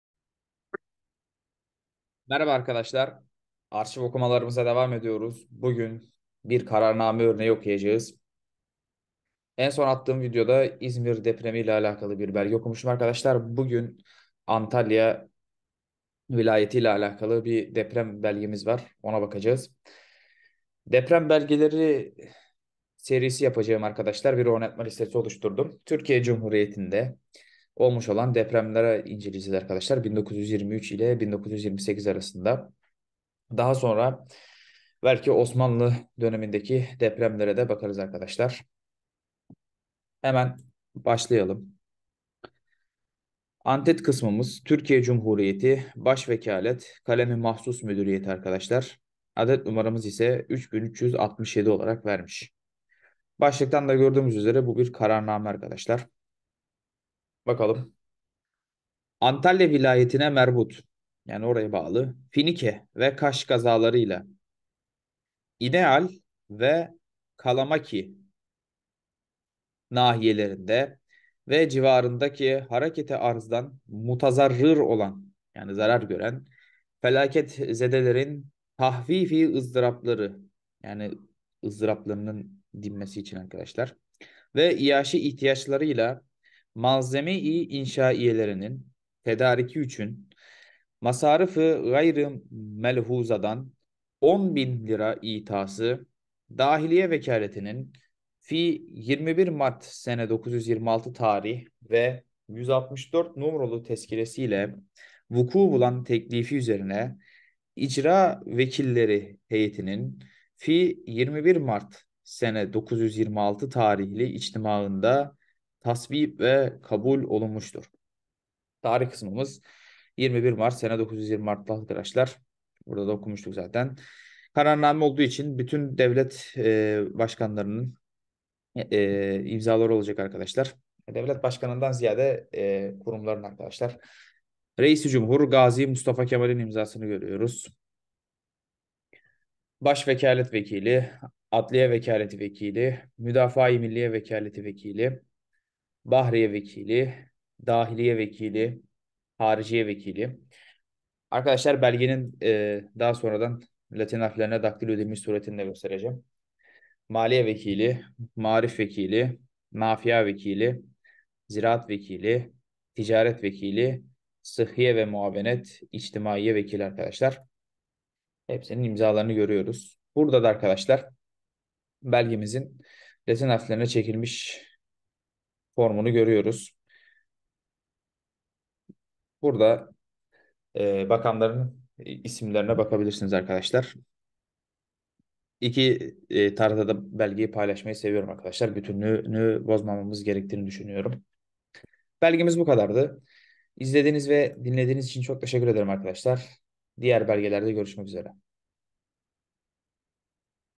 Finike ve Kaş kazaları ile civarında meydana gelen deprem 1926
Belge Özeti :   Finike ve Kaş kazaları ile civarında meydana gelen depremde zarar gören bölge halkına yardım edilmesi.
Yer Bilgisi   :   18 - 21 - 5Dosya Ek   :   99-17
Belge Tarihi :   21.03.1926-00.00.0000
Kurum :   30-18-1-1 / KARARLAR DAİRE BAŞKANLIĞI (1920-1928)
Dili       :   Türkçe